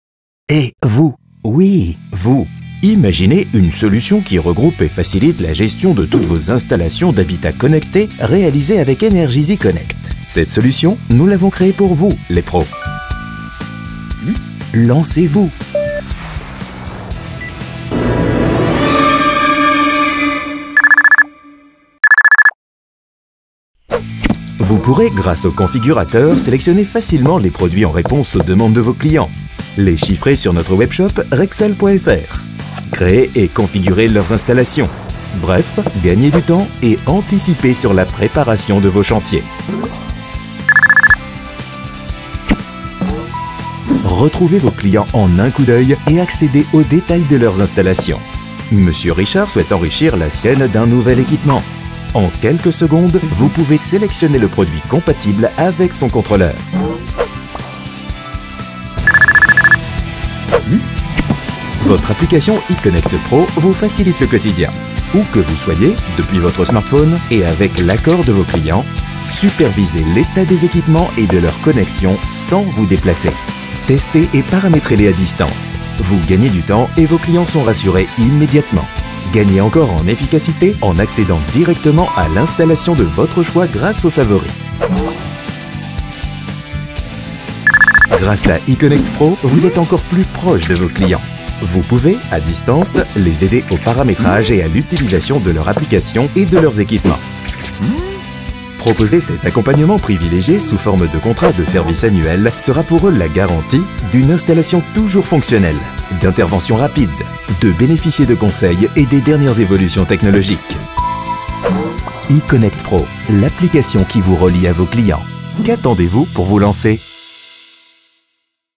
EConnect PRO : L’application dédiée aux pros de l’installation connectée !
Pour compléter son offre de services, Rexel lance EConnect PRO, un outil qui permet aux installateurs de créer, enrichir, superviser et maintenir leurs installations connectées.